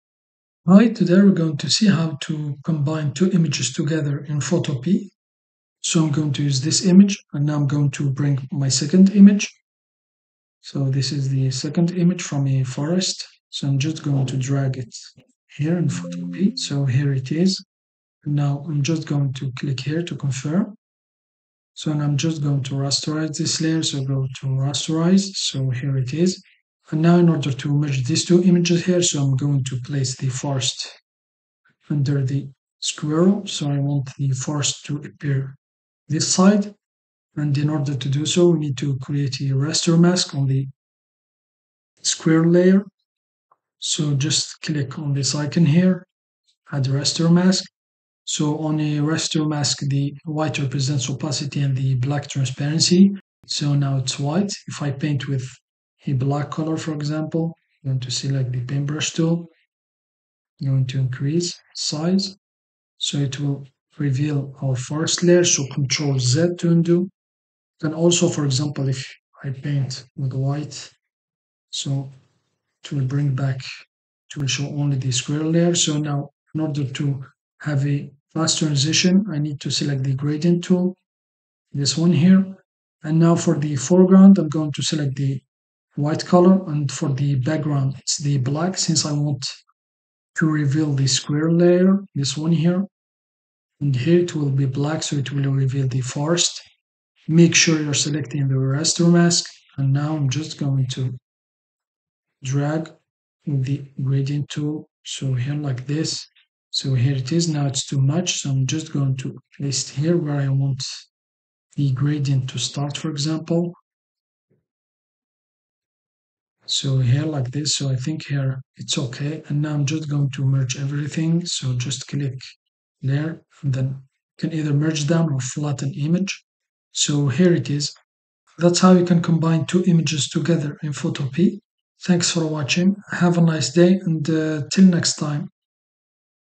How to Combine two Images Together in Photopea
Hi, today in this Photopea tutorial, we are going to learn to Combine two Images Together in Photopea.